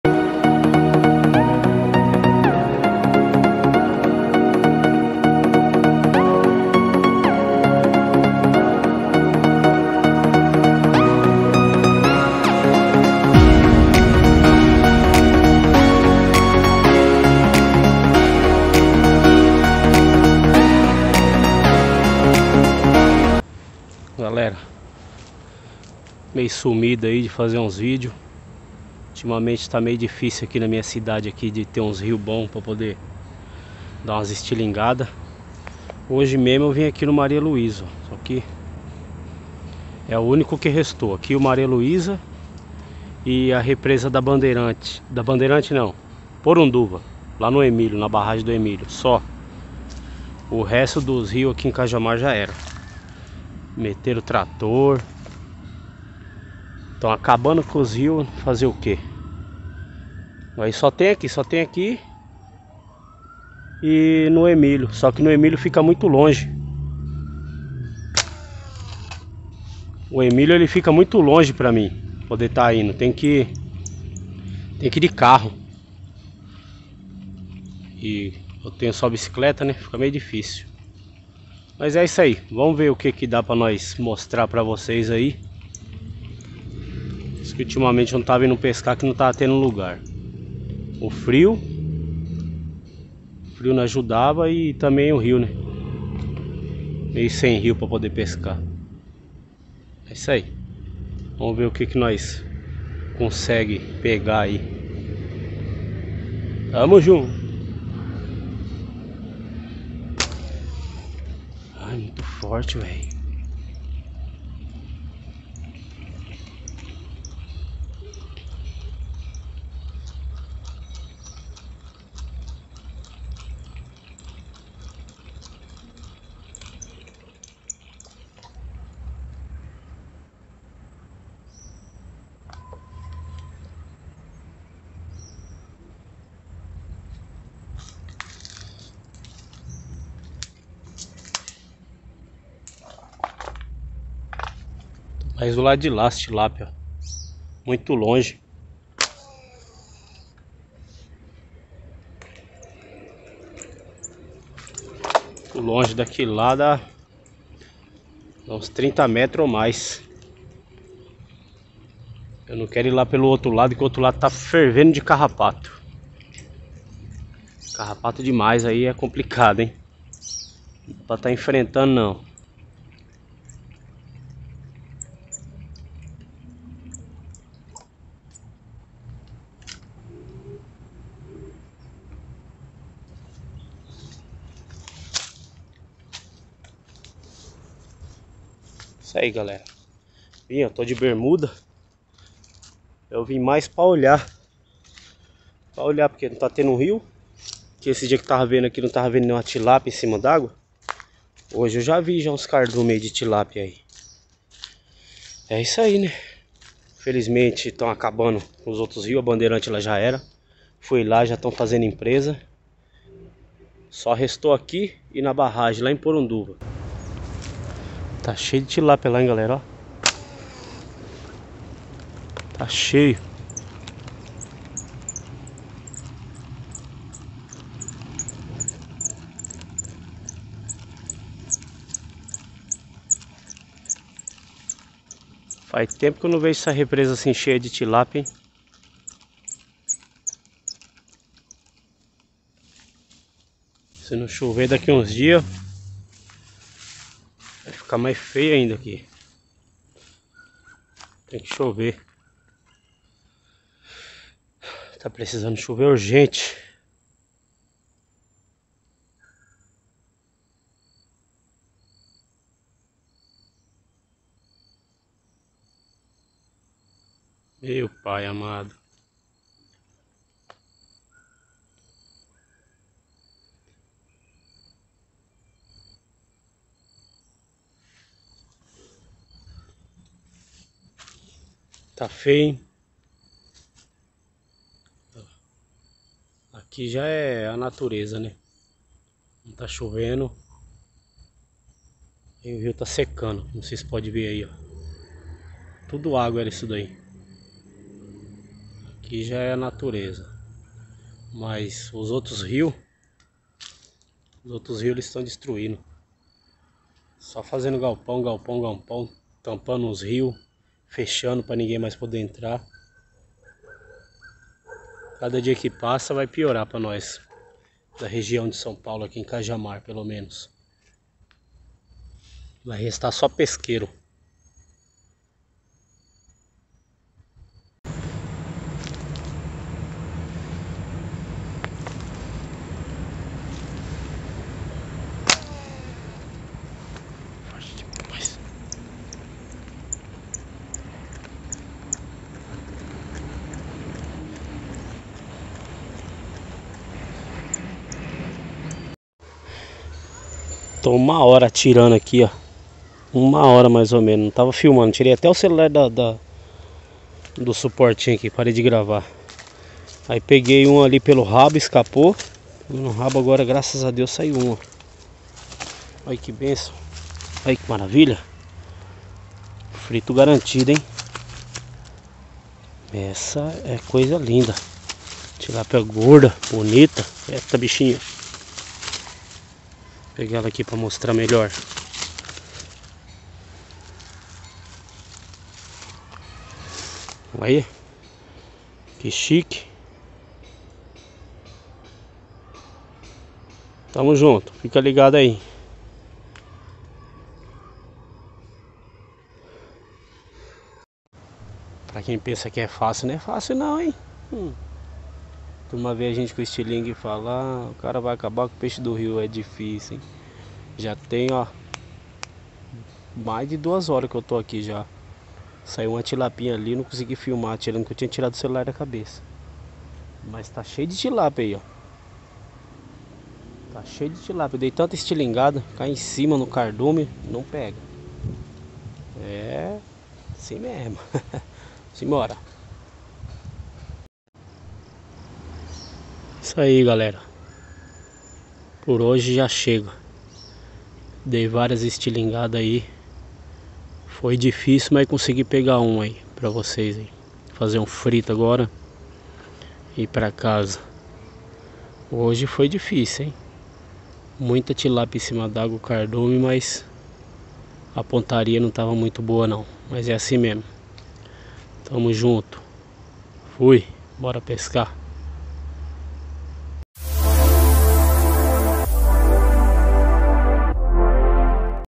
pesca com estilingue.. cada dia q passa vai desanimando agente
pesca com estilingue. cada dia q passa fica mais difícil